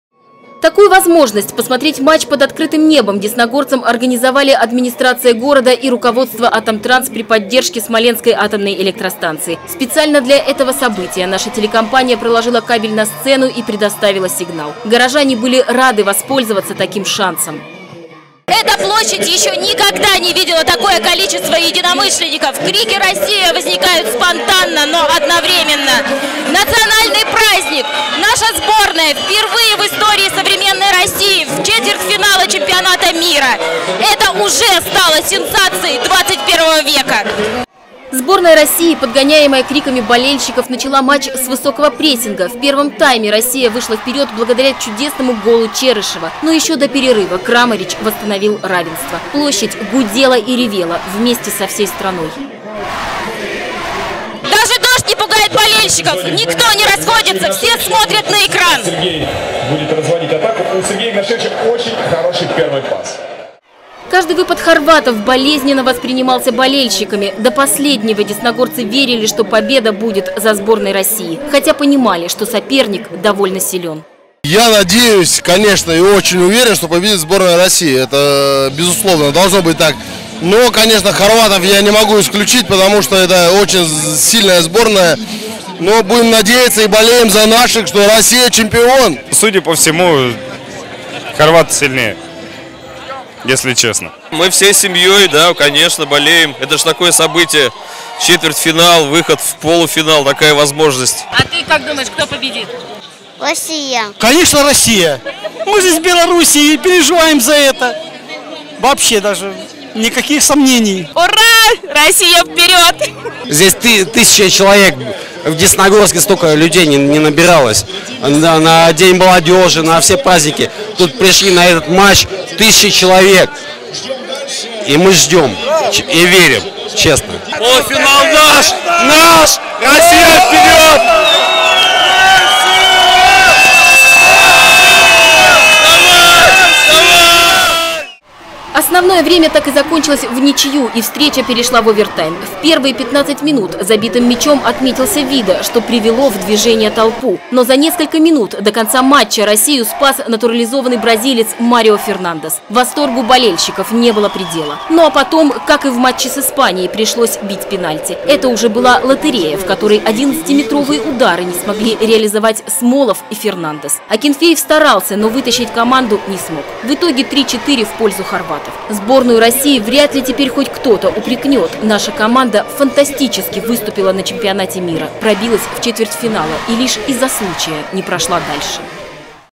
Десна-ТВ: Десногорск поддержал национальную сборную
В субботу 7 июля в Сочи на арене «Фишт» состоялся, пожалуй, главный четвертьфинальный матч чемпионата мира по футболу-2018. В противостоянии сошлись сборные России и Хорватии. Национальную команду поддерживала вся страна. В Десногорске на городской эстраде была организована онлайн трансляция. Более 1000 болельщиков пришли в этот вечер на площадь, чтобы зарядить своей энергетикой российских игроков. Наша съемочная группа стала свидетелем этого фееричного события.